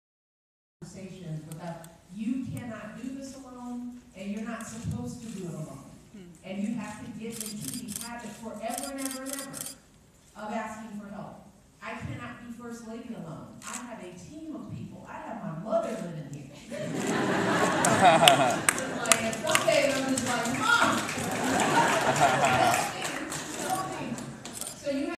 First Lady Michelle Obama "Never be afraid to ask for help"
First Lady Michelle Obama admonishes college-bound students  during a Reach Higher event at the White House to never be afraid to ask for help.

"I've got my mother living with me. Some days I'm like Ma!"

The discussion was moderated by Terrence J. of E! News and included Education Secretary Arne Dunca, DC rapper Wale and Brown University student Manuel Contreras.